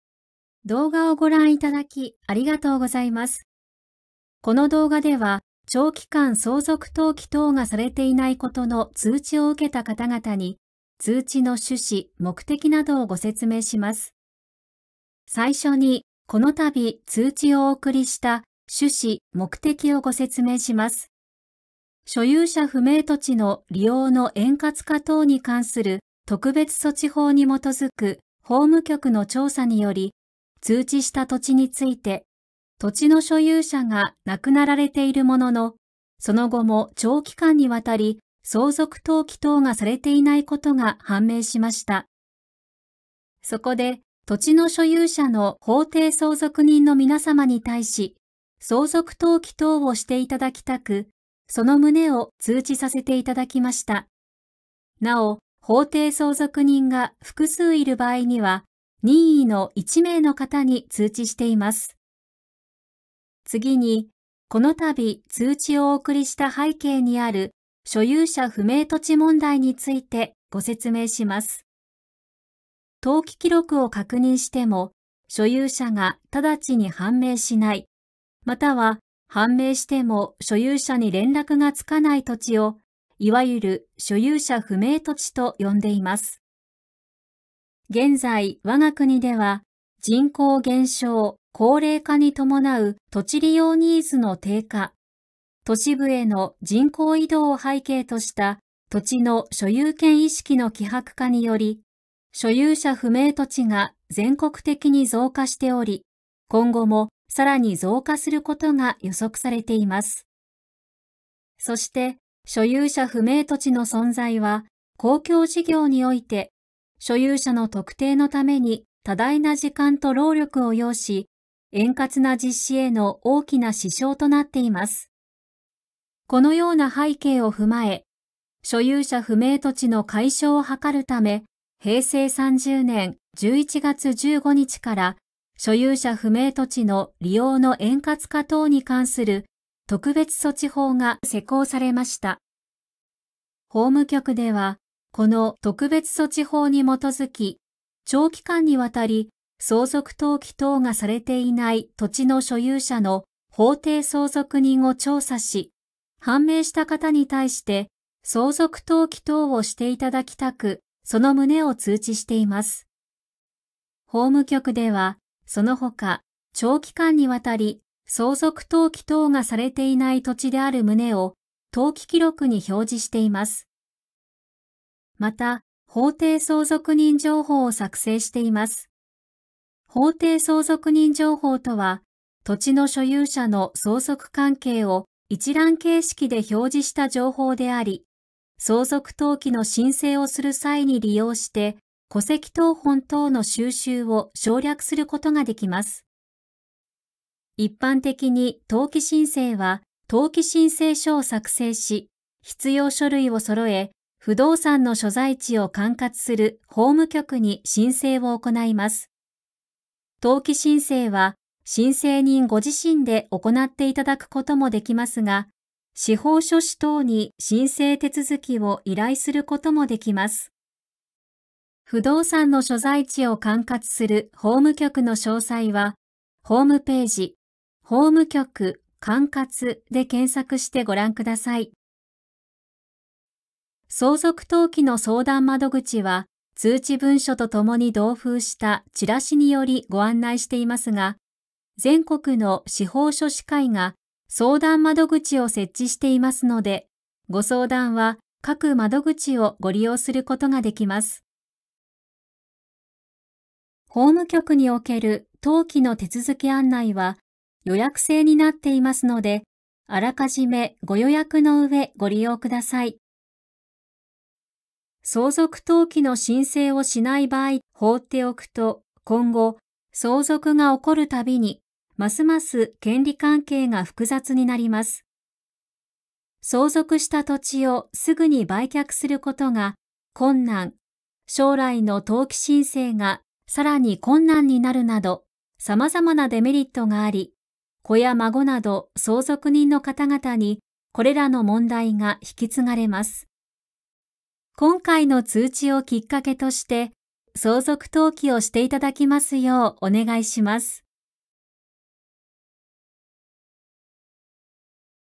長期間相続登記等がされていないことの通知について
法務局では、長期間相続登記がされていない土地について、相続人となる人が誰かを法務局の登記官が調査した上で、登記記録にその旨を記録し、相続関係を一覧化した図である「法定相続人情報」を備え付けることで、公共事業の円滑な遂行に役立てるとともに、直接相続人に通知をして、相続登記を促す事業を行っています。
　この動画では、通知の趣旨・目的や通知を受け取った後の相続登記の申請手続を紹介していますので、是非ご覧ください。